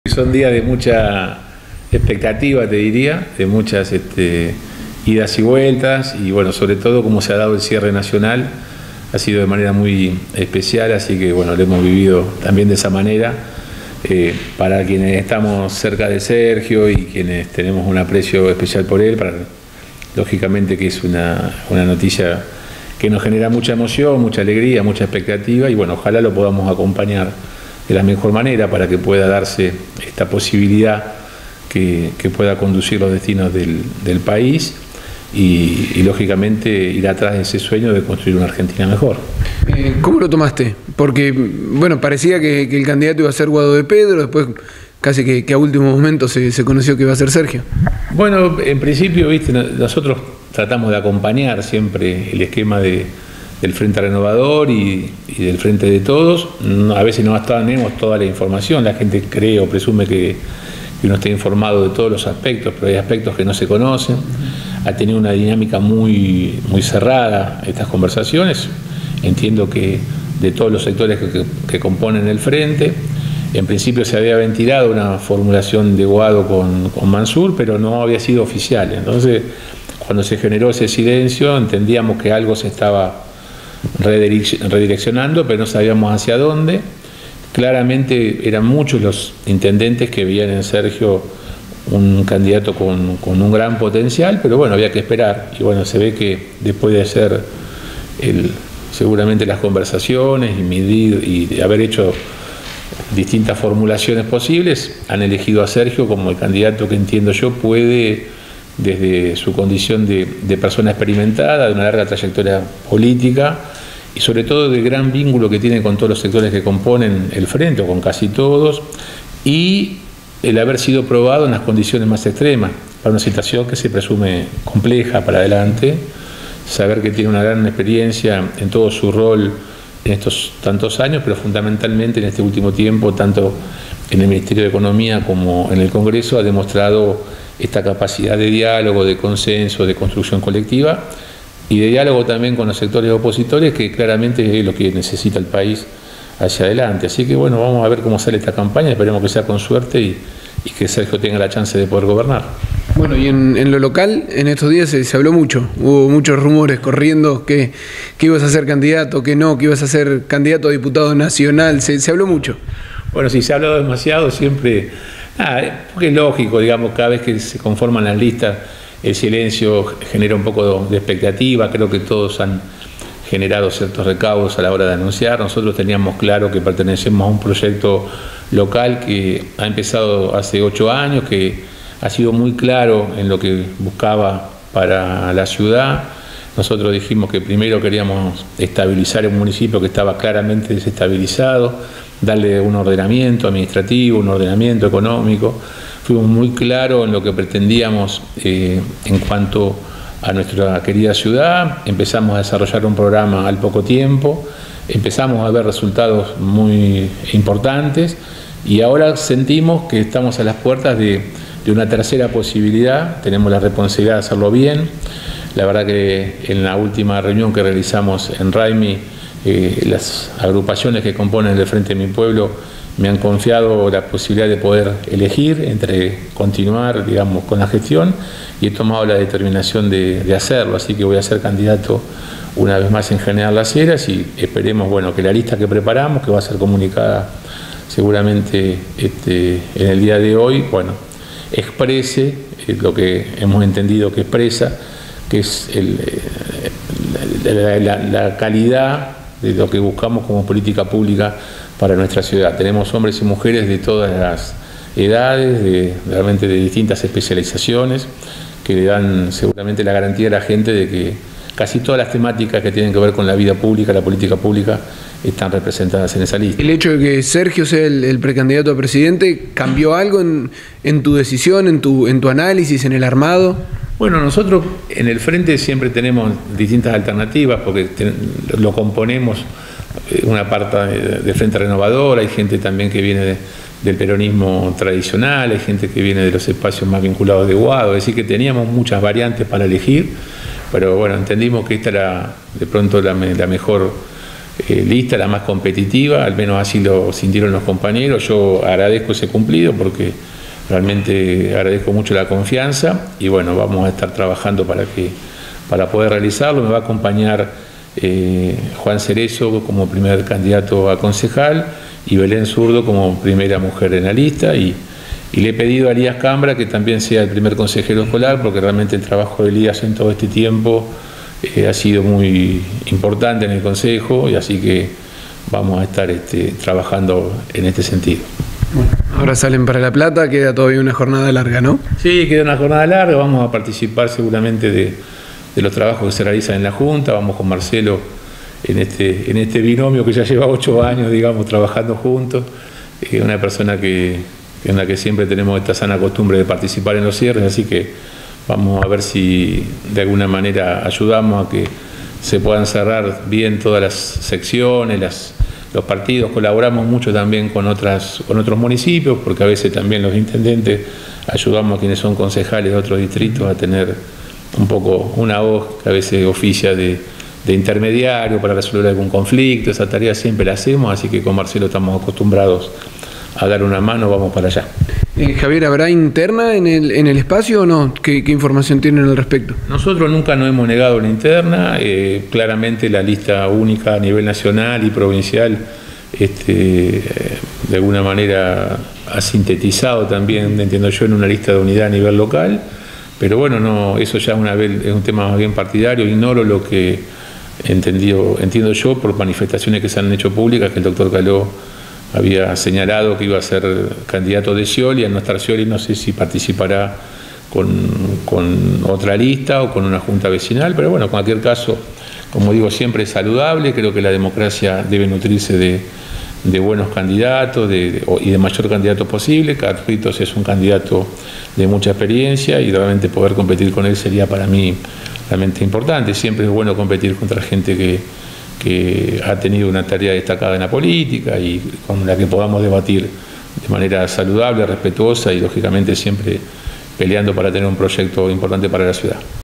Javier Osuna - Precandidato
El actual Intendente confirmó que será precandidato a intendente en las elecciones 2023